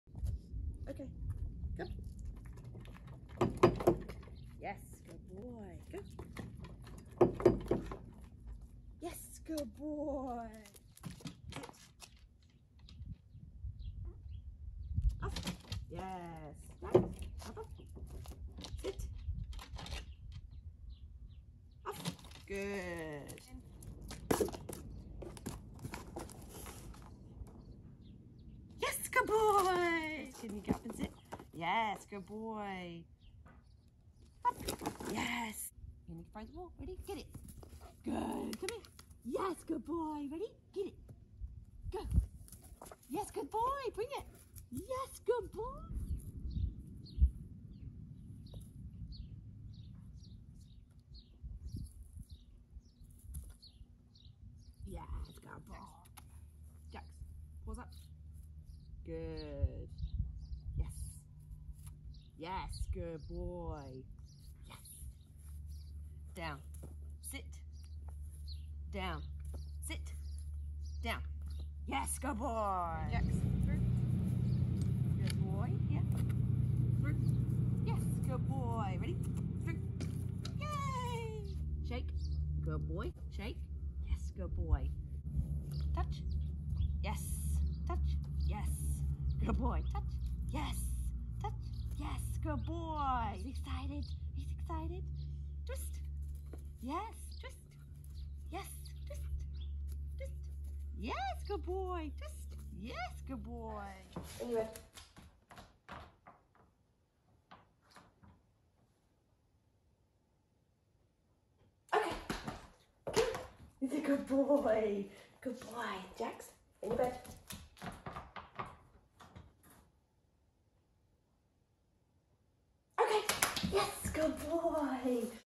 Jax’s AKC novice trick title entry
1. Walk along a board
2. place on a table
3. Sit in a box
4. Sit on a box
5. Fetch and retrieve
6. Hand signals up and down
7. front paws on arm
8. sit and Down x3
9.through a hoop
10. shake
11. Touch
12. Spin both ways
13. Kennel Up